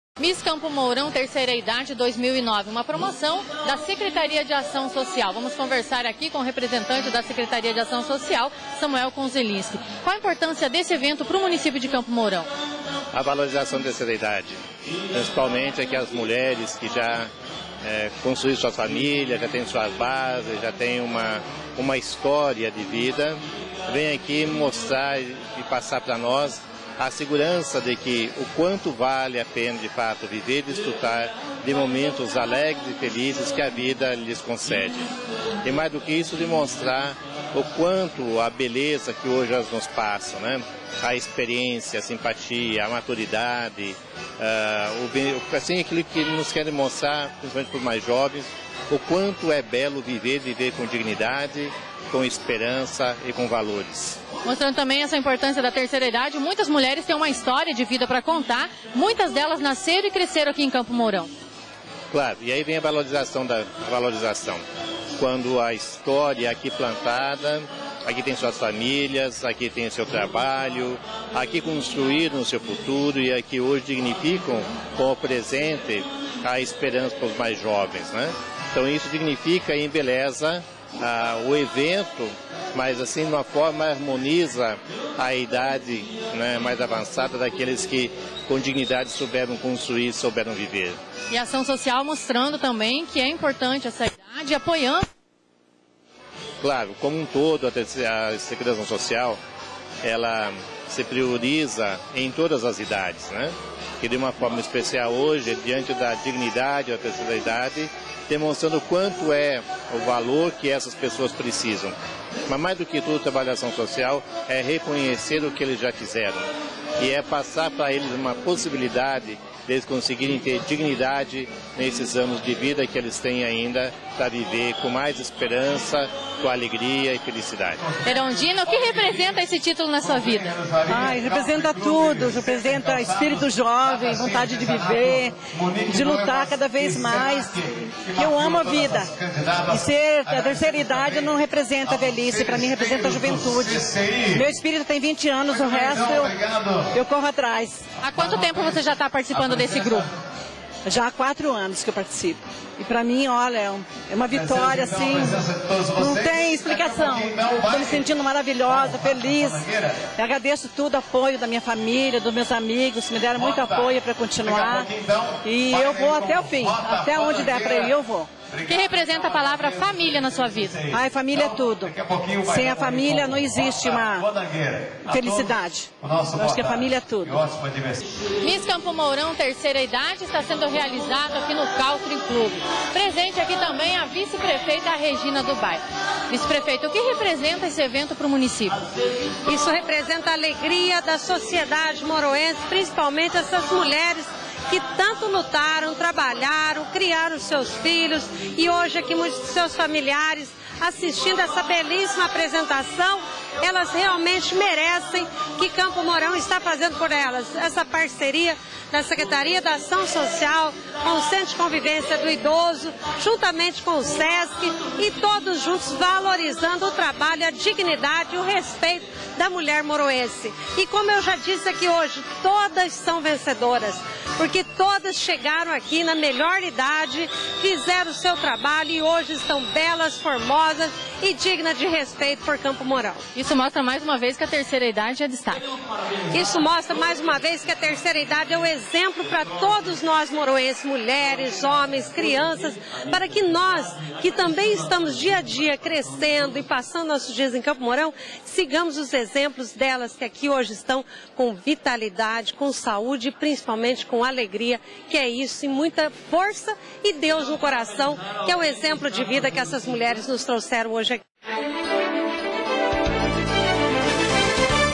Tv em Ação - Miss Campo Mourão Terceira Idade
A secretaria de Ação Social realizou o evento que escolheu a Miss Campo Mourão 3ª Idade 2009